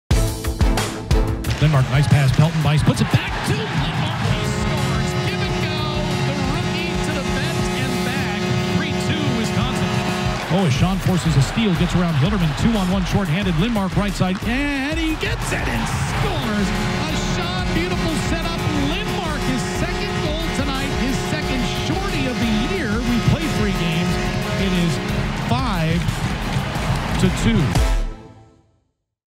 Owen Lindmark Goals vs UMD
Owen Lindmark scored two goals Friday night against Minnesota Duluth. Find out more about the freshman and the day in the life of a Badger hockey player